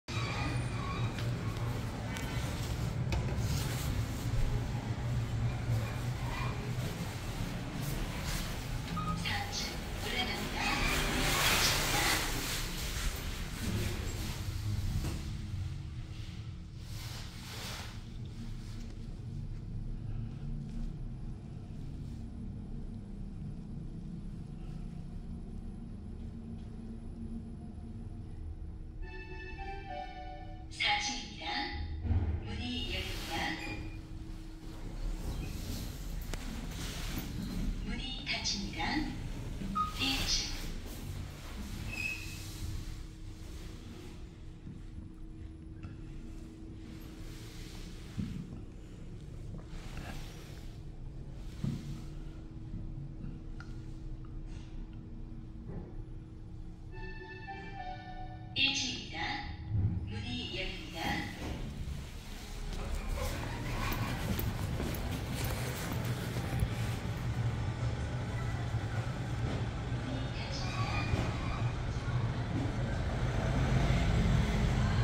[4K 엘리베이터 탑사기] 광주광역시 광산구 월계동 삼라프라자 현대엘리베이터 WBLX-GT-2(M)
광주 첨단지구 첨단동아아파트 옆에 있는 대형 상가 겸 주차빌딩입니다. 국가승강기정보센터 승강기정보 상에 등록된 건물명은 '삼라주차빌딩'입니다.

▷승강기 소개
엘리베이터는 최초설치 당시 현대엘리베이터의 1998년식 WC-VF를 운용했다가, 2021년 말 동사의 기어리스 모델 WBLX로 전면교체됐습니다. 권상기는 GT권상기를 사용했습니다.

구축아파트 엘리베이터 교체분에서 주로 적용되는 황토색 디자인이 채택되었고, STN-LCD 타입의 '스마트 인디게이터' 1세대 위치표시기가 적용됐습니다. 용량은 매우 넓은 1600kg/21인승으로, 주행 중 별 다른 이상은 없었습니다.

승강기 고유번호: 7000-547
설치시기: 2021년 12월
 * 교체전: 1998년 2월
교체공사여부: O
용도: 승객용
모델: WBLX-GT-2(M)
 * 교체전: 현대 WC-VF
형식: 권상식 - VVVF
위치표시기 TYPE: STN-LCD
정원: 21인승
적재하중: 1600kg
운행구간: B1 ~ 4층
정격 속력: 60m/min